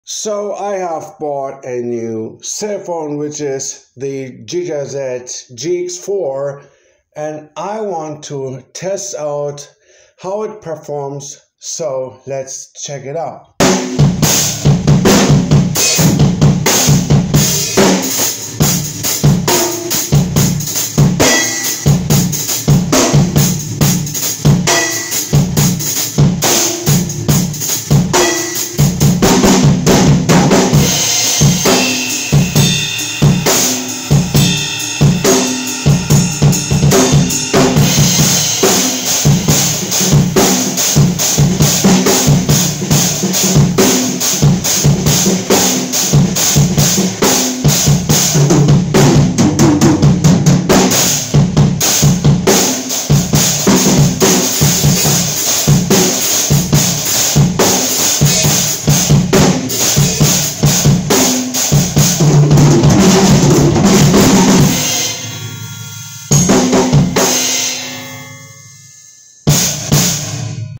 Gigaset GX4 Sound Test
i bought a new cellphone! it will be used in future videos